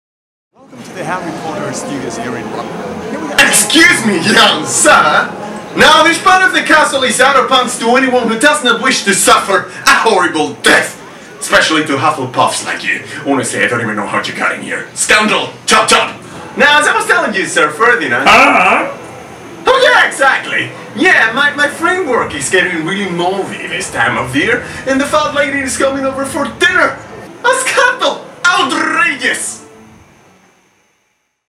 An Unusual Interview at Hogwarts
A strange and unexpected interview took place in Warner Bros' Harry Potter studios in London, while our crew was recording a documentary about Hogwarts and their talking portraits. Sir Ferdinand is a star. Not suitable for Hufflepuffs.

This unusual hogwarts interview took place inside the castle, it is obviously fan made and we just wanted to have a laugh with this particular Hogwarts reunion. We met some interesting new Hogwarts characters behind the cameras and they gave us a tour of the forbidden areas.

Hogwarts interview, fanmade, talking paintings